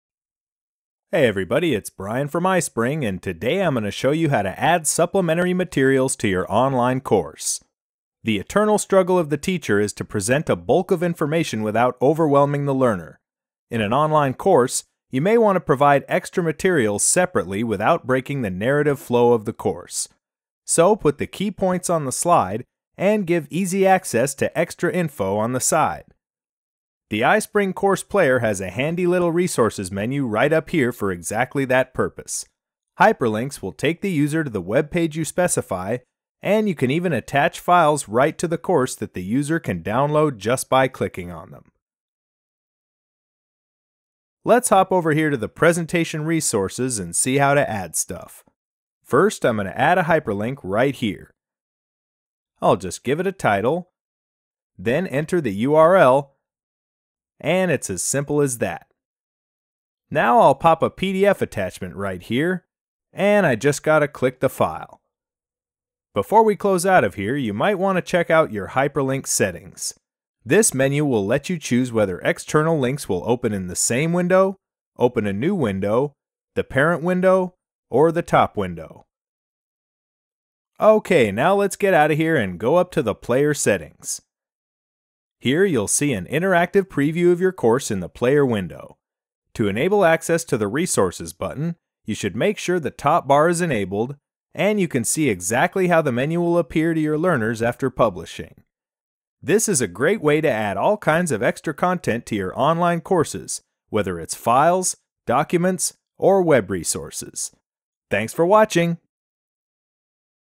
Adding Resources to Your Online Course
This tutorial shows you how to add supplementary resources like file attachments and hyperlinks to your online course.